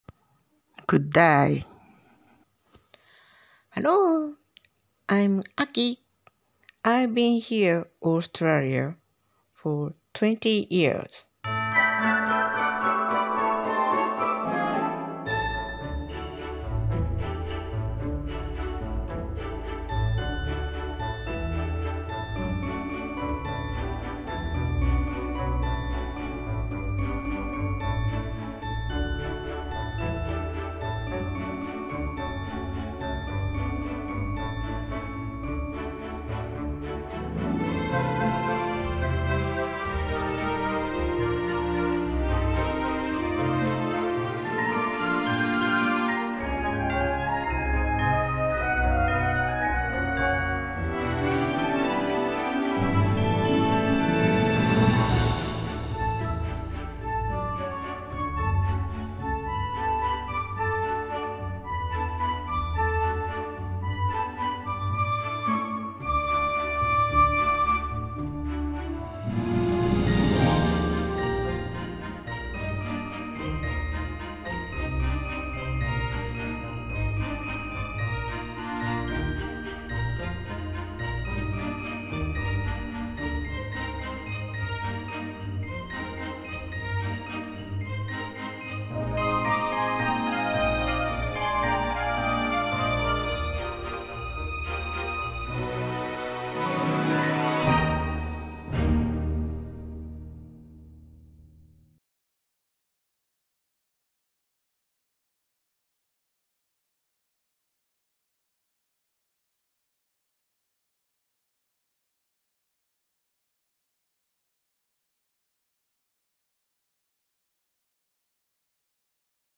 DAY from Australia🦘オーストラリアから グッダイ🐨
初回は11～12月に咲く二種類の 花です
Known? I would like to start introducing Australia little by little.
First, two kinds of flowers that bloom from November to December.